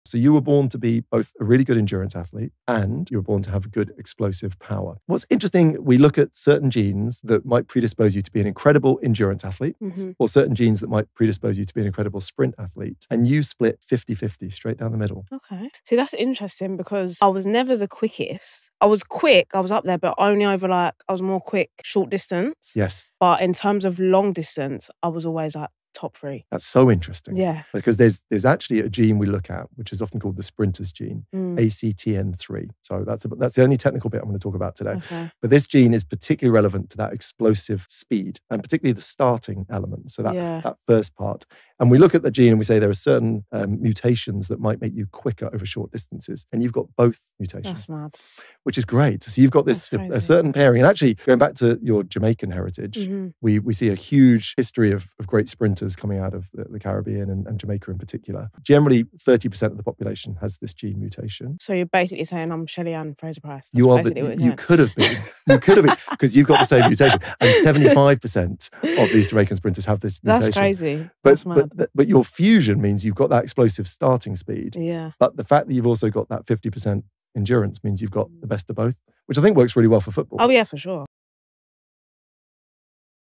The Sprinter’s Gene Explained 🧬
Did you know there’s actually a sprinter’s gene? 🧬 Oli Patrick explains how genetics can influence endurance vs explosive power, and why having both can be such an advantage.
🎧 Listen wherever you get your podcasts▶️ Watch on YouTube